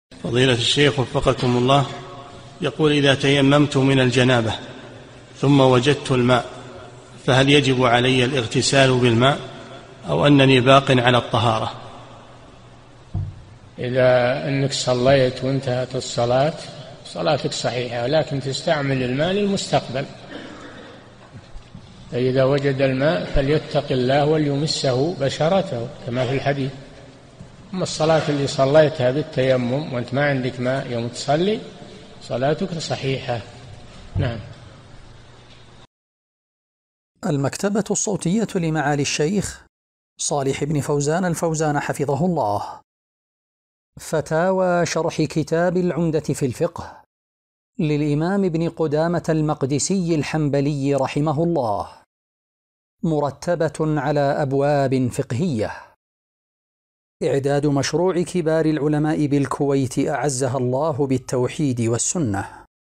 [295 -3022] إذا تيممت من الجنابة ثم وجدت الماء فهل يجب علي الاغتسال بالماء؟ أو أنني باق على الطهارة؟
-"[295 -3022] السؤال: يقول: إذا تيممت من الجنابة ثم وجدت الماء فهل يجب علي الاغتسال بالماء؟ أو أنني باقٍ على الطهارة؟
الجَوَابُ: إذا أنك صليت وانتهت الصلاة صلاتك صحيحة لكن تستعمل الماء للمستقبل، فإذا وجد الماء فليتق الله وليمسه بشرته""، كما في الحديث، أما الصلاة التي صليتها بالتيمم وأنت ما عندك ماء يوم تصلي، صلاتك صحيحة، نعم.
للاشتراك في قناة الشيخ صالح الفوزان

للاستفسار والإقتراحات والدعم
رئيس الفريق
لمشروع كبار العلماء